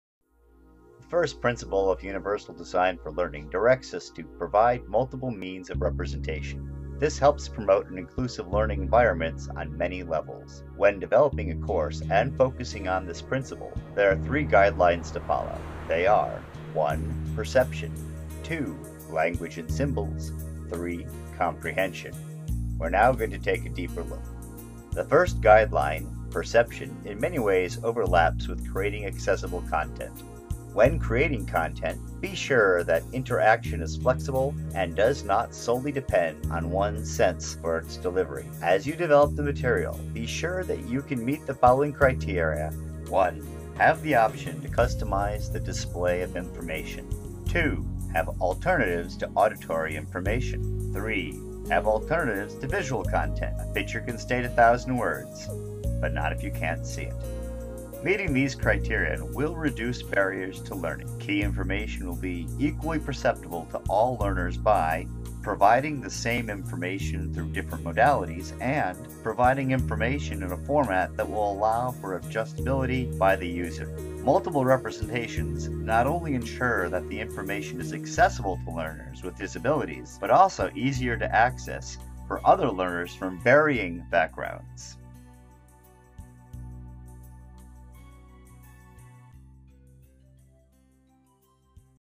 UDL - Principle of Representation Guideline 1 - Perception
This video briefly covers the first guideline of the Principle of Representation in Universal Design for Learning.